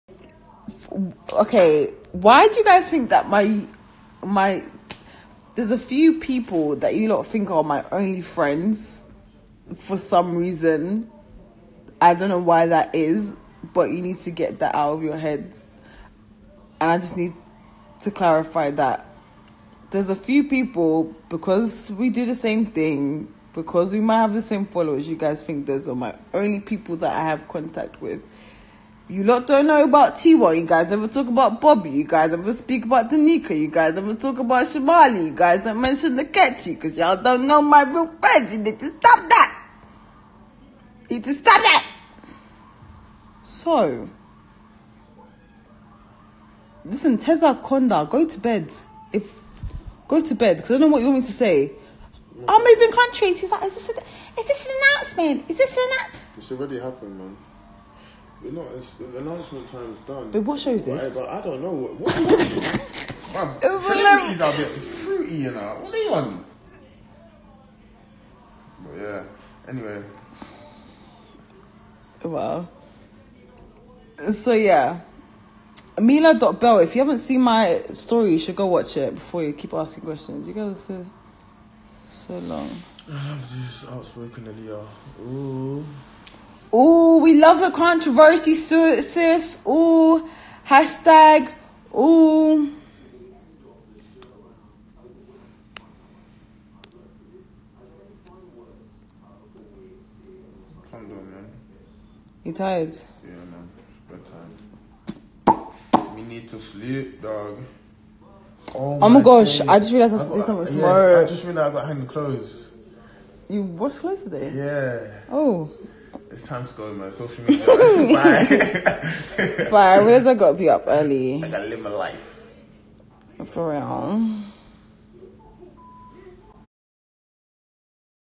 ALIYAHMARIABEE SAYS HER YOUTUBE FRIENDS NELLA ROSE & THE PATRONNES ARE NOT HER REAL FRIENDS ON INSTA
AliyahMariaBee hasn’t been seen with her fellow influencer and YouTuber friends such as Nella Rose, Adeola Patronne, Sola Patronne, Mimi Missfit and Annie Drea in a while, and although we are yet to confirm if there is any beef between them, it does sound like Aliyah was being shady by suggesting that the other people she mentioned in the Instagram live video are her real friends.

Aliyah is now married and expecting a baby so maybe she prefers to do her own thing with her husband now? What do y’ll think?